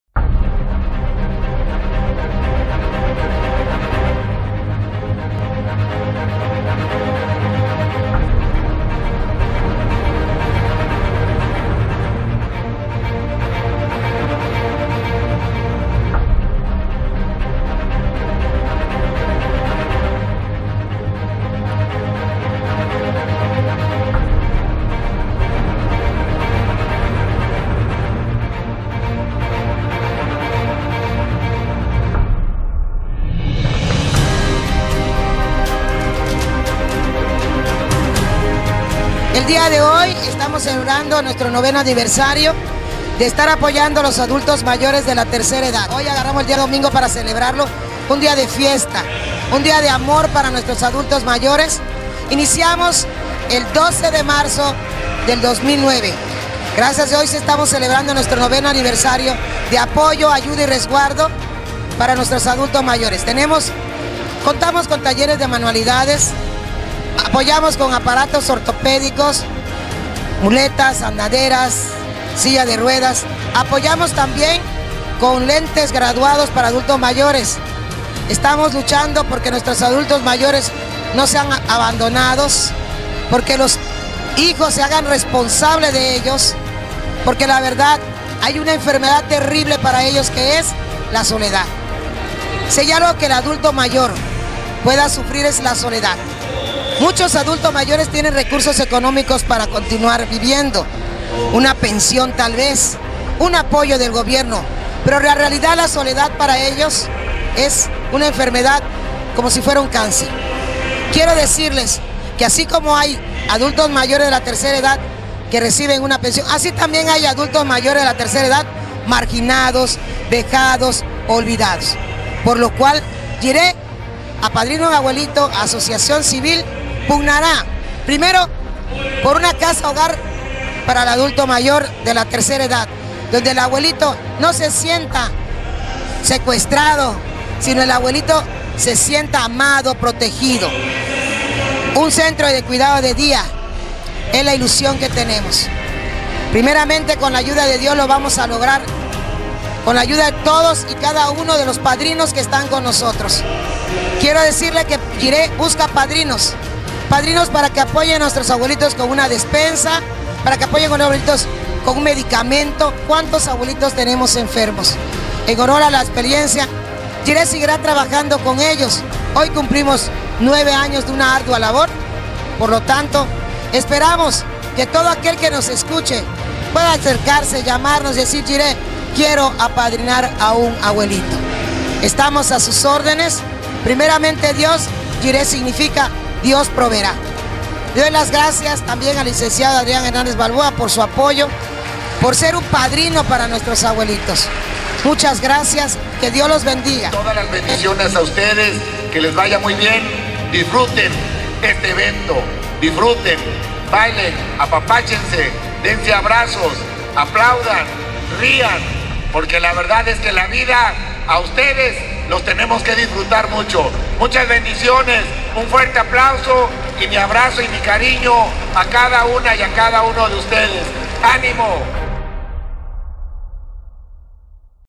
9 Aniversario, Yireh Apadrina un Abuelito A.C.
FESTEJA SU NOVENO ANIVERSARIO LA ASOCIACIÓN CIVIL YIREH APADRINA UN ABUELITO. MÚSICA, REGALOS Y DELICIOSO COMIDA FUE PARTE DEL AGASAJO QUE TUVIERON LOS ABUELITOS.